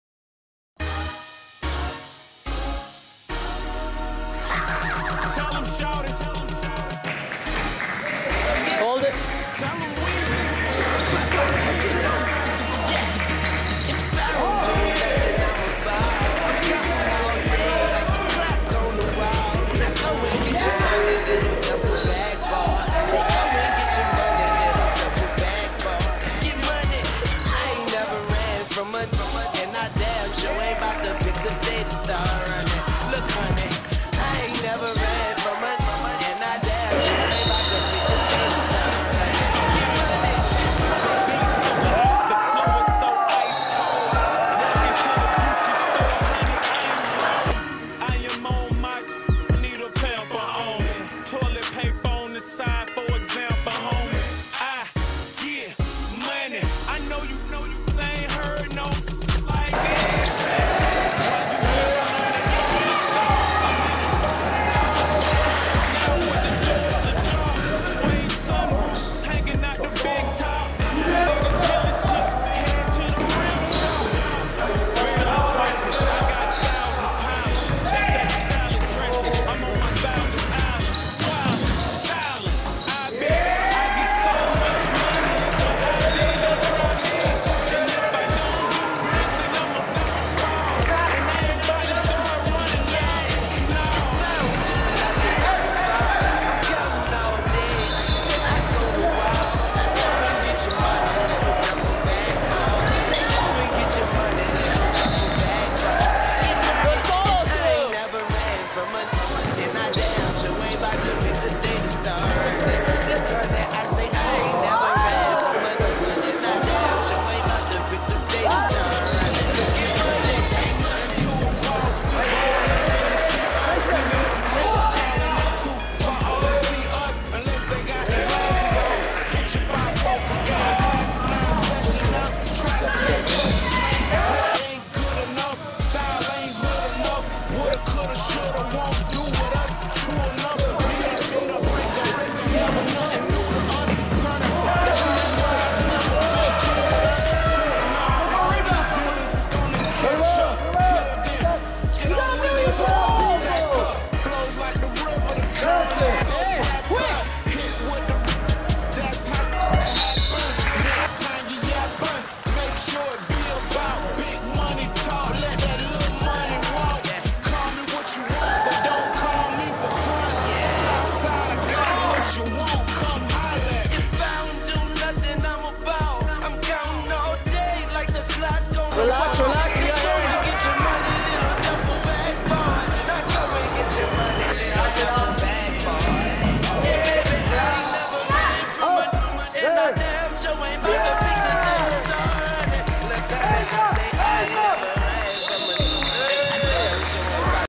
Luke Galati - Mixtape
Luke Galati 6'0 Point Guard from Toronto, Ontario attends Riverdale Collegiate

Highlights from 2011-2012 OBA tournaments with the East York Warriors